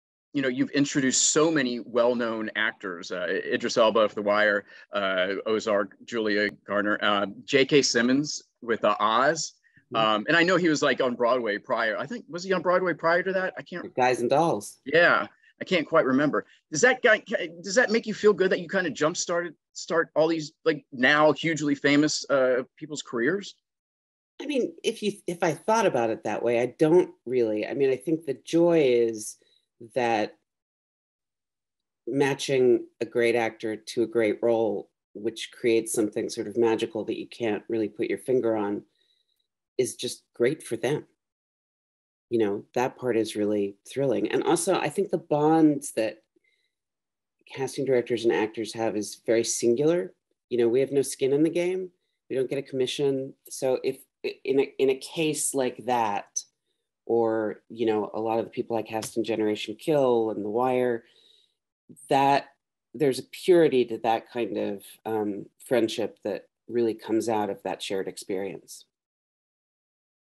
Casting Director Alexa Fogel on Helping to Start Actors Careers | Daily Actor Interview Snippet
Casting Director Alexa Fogel on helping start actors careers and the joys of casting.